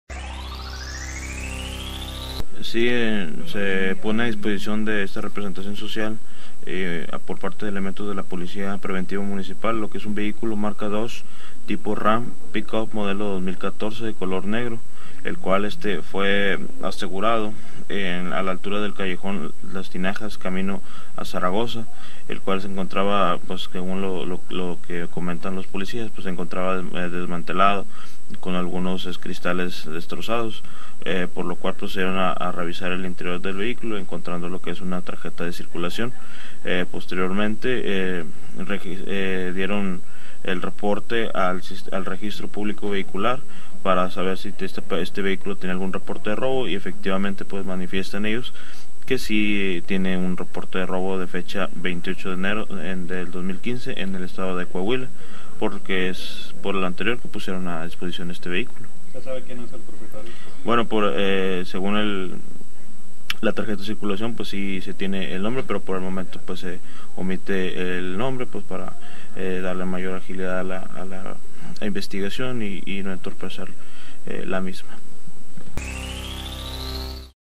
12 NOTICIAS Ubicaron una camioneta la cual contaba con reporte de robo
12 NOTICIAS.-
PIEDRAS NEGRAS.,COAHUILA. Elementos municipales ubicaron una camioneta la cual contaba con reporte de robo, se trata de una pick up Dodge RAM modelo 2014 negra la cual estaba prácticamente desmantelada, la unidad se encontraba sobre el camino a las tinajas, ya se efectúan las investigaciones, así lo informa Joel Reyes ministerio público asistente del primer turno.

F. Mario Meza